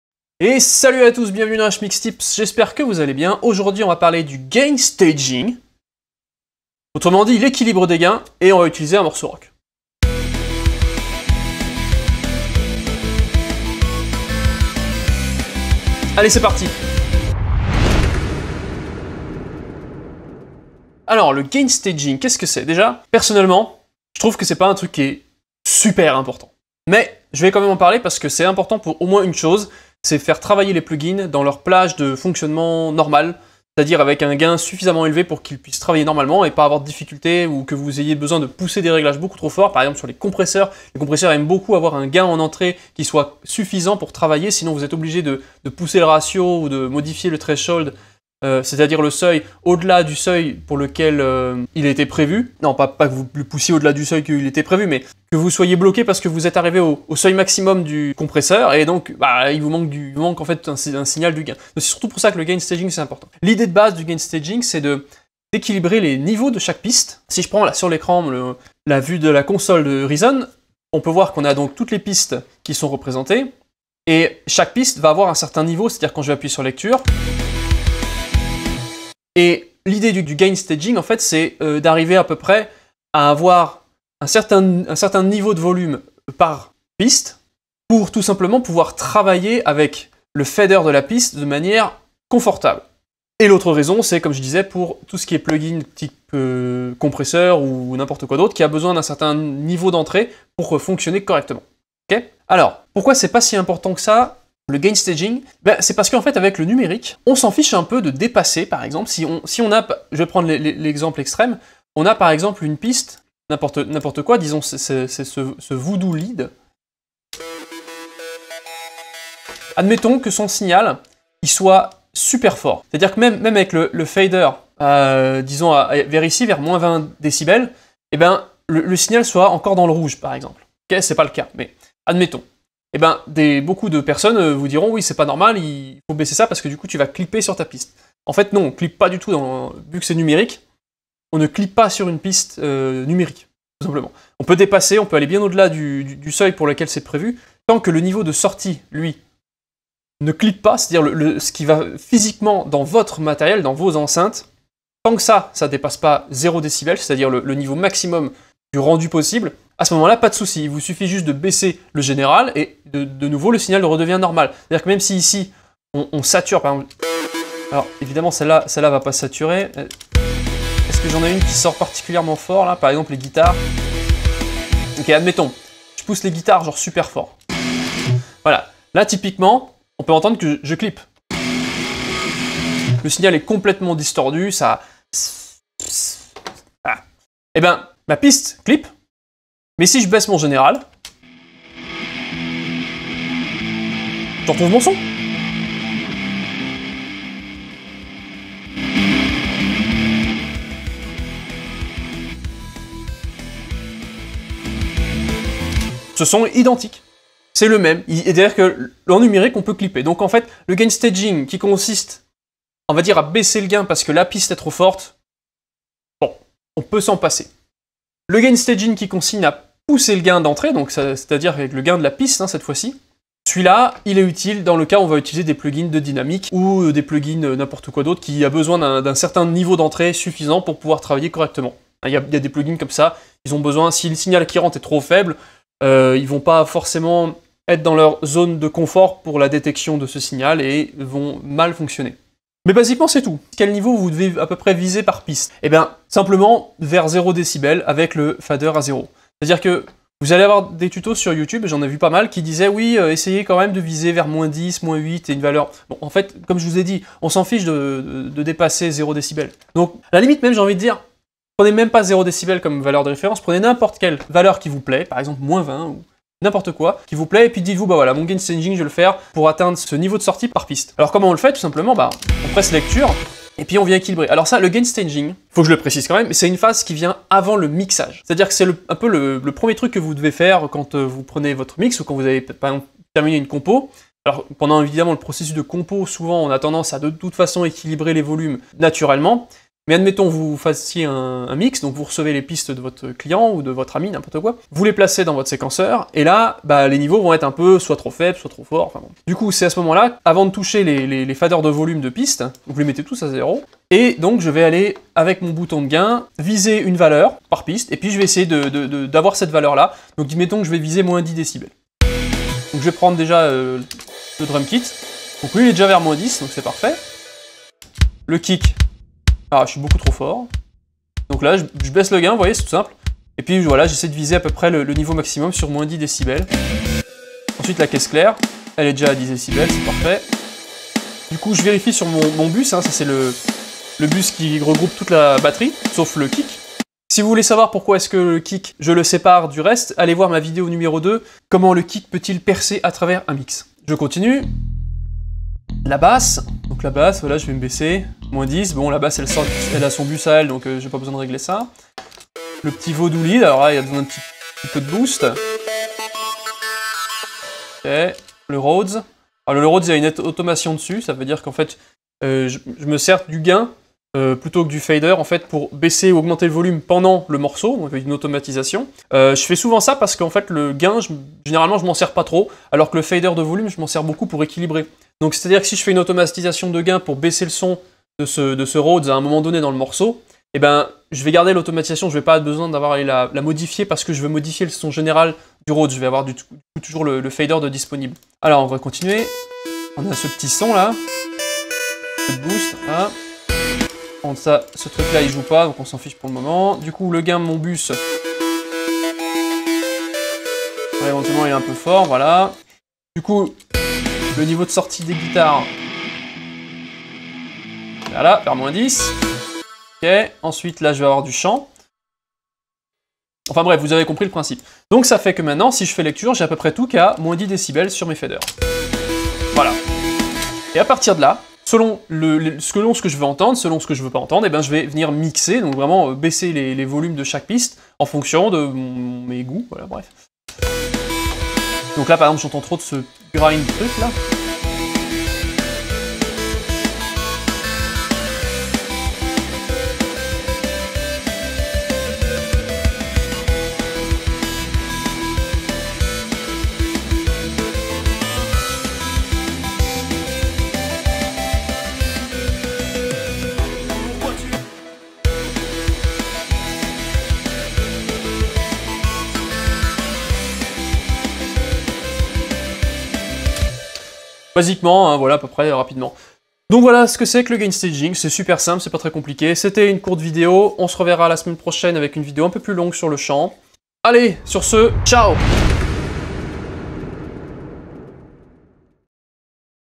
[MIX TIPS] Gain Staging - Equilibre des gains
Aujourd'hui le Gain staging, chose dont j'ai beaucoup entendu parlé sur internet et mon avis sur ce sujet.